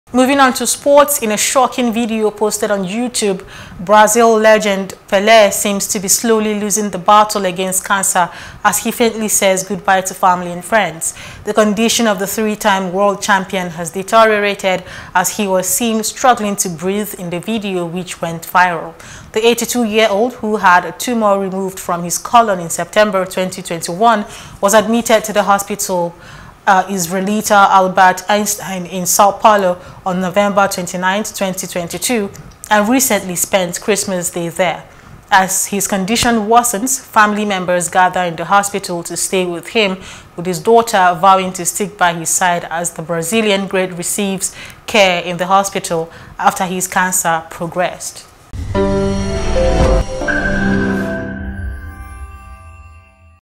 Pele Bids Family Farewell In Shocking Video
In a shocking video posted on YouTube, Brazil legend, Pele, seems to be slowly losing the battle against cancer as he faintly says goodbye to family and friends.